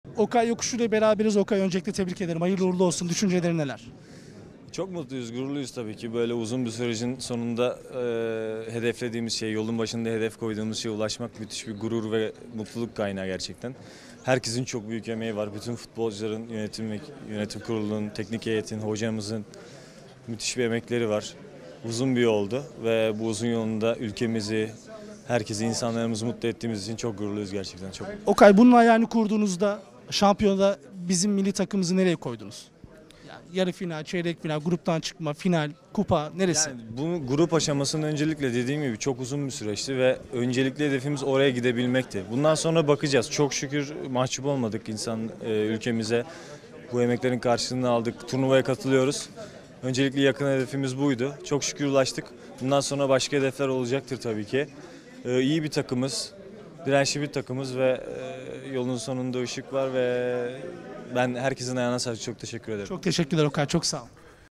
Okay Yokuşlu, Türkiye 0-0 İzlanda Maçı Sonrası Tivibu Spor'a Konuştu
A Milli Takım'ın yıldız ismi Okay Yokuşlu, Euro 2020'ye gitmeye hak kazanılmasının ardından Erkut Öztürk'ün sorularını yanıtladı.